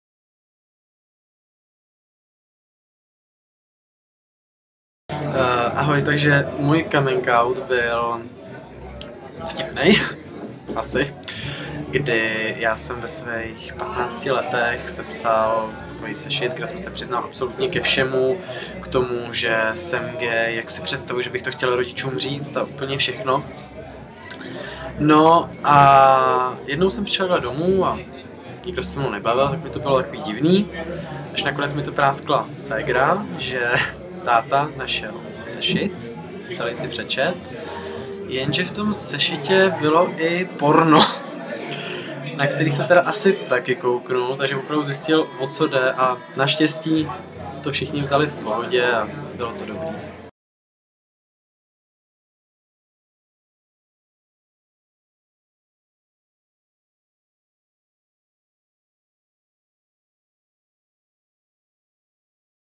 Coming out - Michal Pařízek
Hlavním tématem Prague Pride 2013 je coming-out. Naše zvědavá kamera se ptá na různých LGBT akcích "Jak probíhal ten váš"?